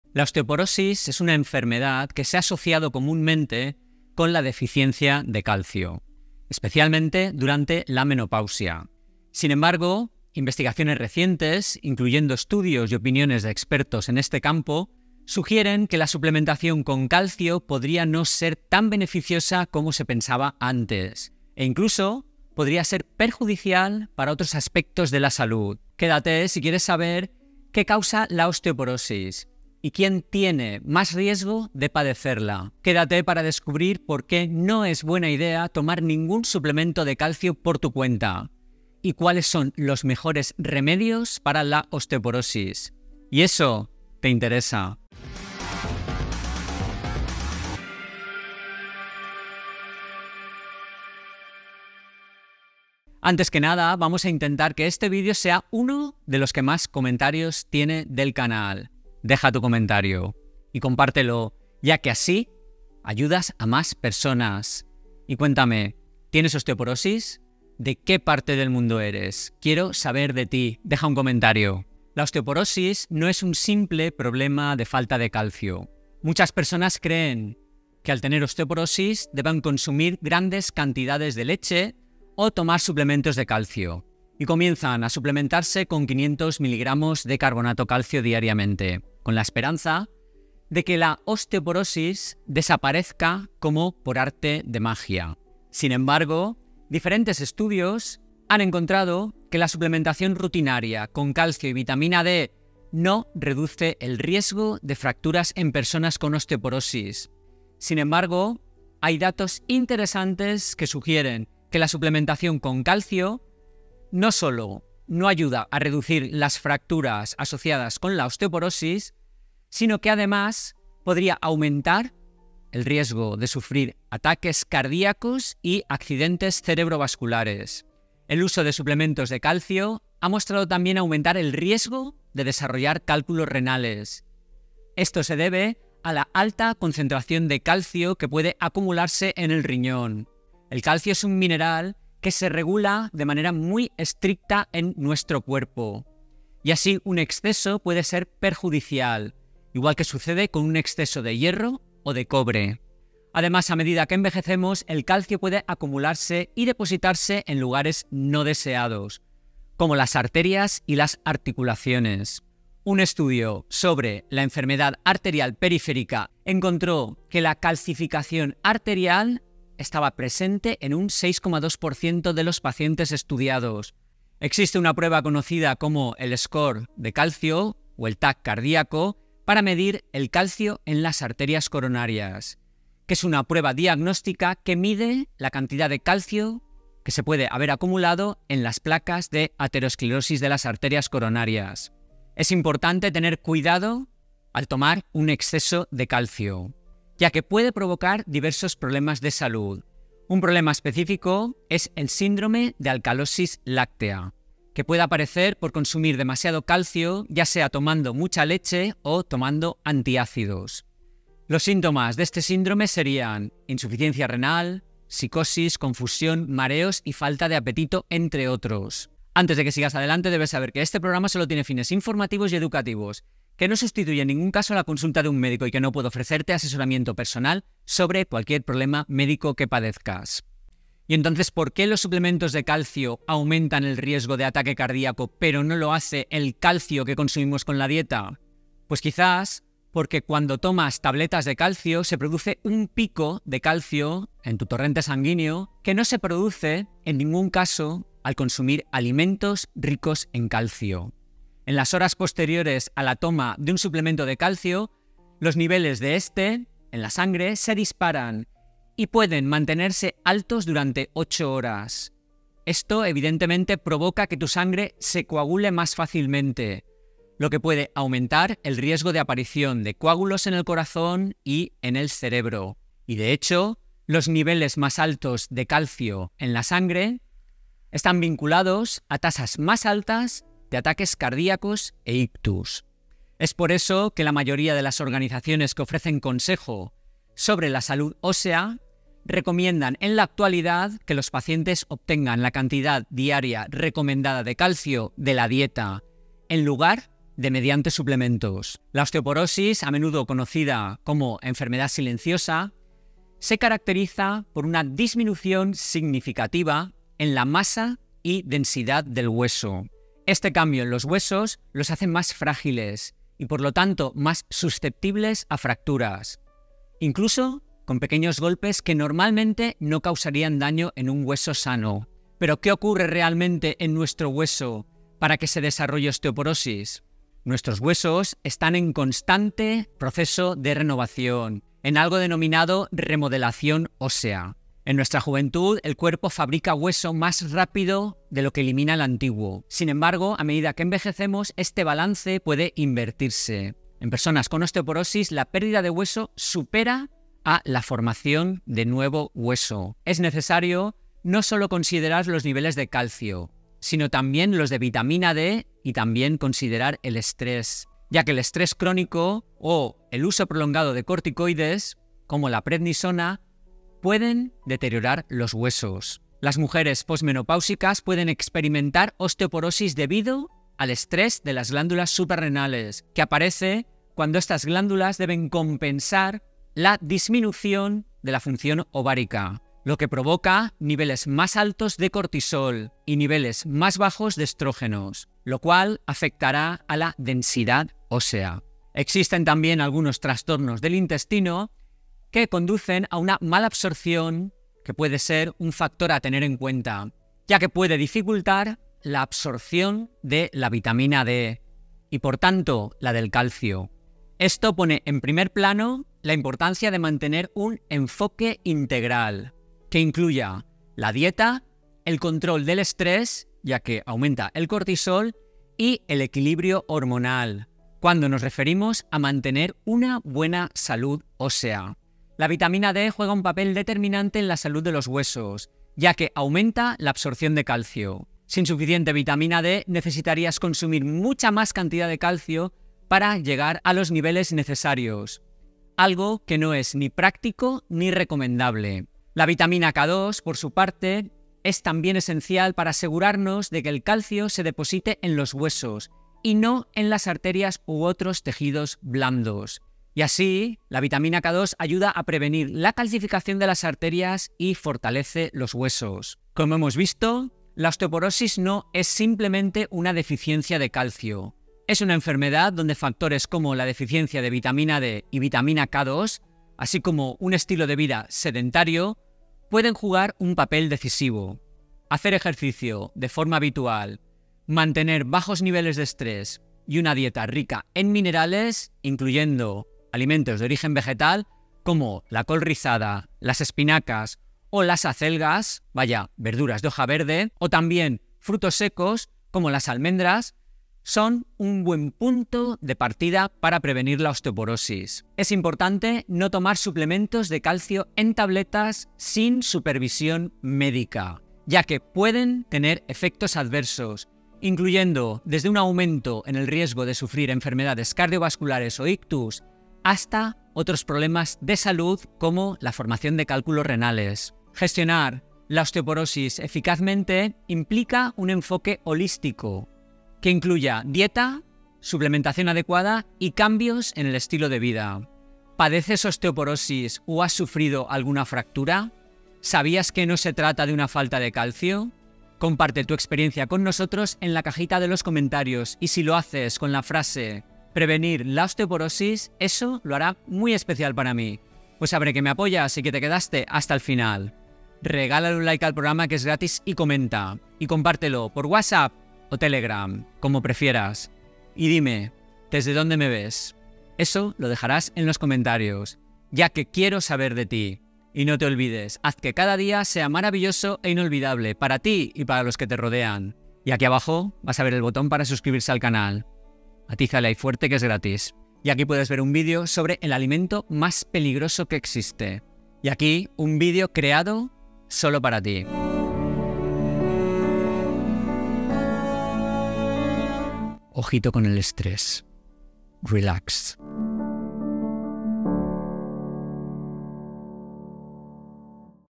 😳 La OSTEOPOROSIS 🦴 no es una DEFICIENCIA de CALCIO - Remedios Osteoporosis
Descubre la verdad sobre la relación entre la osteoporosis y el calcio. Aprende cómo prevenir esta enfermedad ósea y por qué la suplementación no siempre es la mejor opción.

00:00 INTRODUCCIÓN: La OSTEOPOROSIS no es una DEFICIENCIA de CALCIO
01:35 Suplementos pueden dañar
03:50 Riesgos de suplementos
04:58 Osteoporosis: causa y tratamiento
07:36 Prevención holística

La osteoporosis no es solo una cuestión de falta de calcio. La suplementación con calcio podría no ser tan beneficiosa como se pensaba antes. Tomar suplementos de calcio no reduce el riesgo de fracturas asociadas con la osteoporosis y puede aumentar el riesgo de ataques cardíacos, accidentes cerebrovasculares y cálculos renales. El exceso de calcio puede acumularse en lugares no deseados como las arterias y las articulaciones, lo que podría aumentar el riesgo de enfermedades cardiovasculares.

El síndrome de alcalosis láctea puede ocurrir por consumir demasiado calcio, ya sea a través de la leche o de antiácidos. La ingesta excesiva de calcio a través de suplementos puede provocar problemas de salud como insuficiencia renal, psicosis, confusión, mareos y falta de apetito.
Recomendaciones:

La mayoría de las organizaciones de salud ósea sugieren obtener la cantidad diaria recomendada de calcio a través de la dieta en lugar de suplementos. El enfoque holístico para prevenir y tratar la osteoporosis incluye dieta, suplementación adecuada y cambios en el estilo de vida, como ejercicio regular y manejo del estrés.

Además de la deficiencia de calcio, la osteoporosis puede estar relacionada con la deficiencia de vitamina D, vitamina K2, estrés crónico, uso prolongado de corticoides y trastornos del intestino que afectan la absorción de nutrientes.

Mantener un estilo de vida activo, gestionar el estrés y consumir una dieta rica en minerales, incluyendo alimentos como verduras de hoja verde, frutos secos y alimentos fortificados con calcio y vitamina D, puede ayudar a prevenir la osteoporosis.

Se recomienda evitar tomar suplementos de calcio en tabletas sin supervisión médica debido a posibles efectos adversos, como un mayor riesgo de enfermedades cardiovasculares, accidentes cerebrovasculares y formación de cálculos renales.
Información legal.
Vídeo de grabación propia.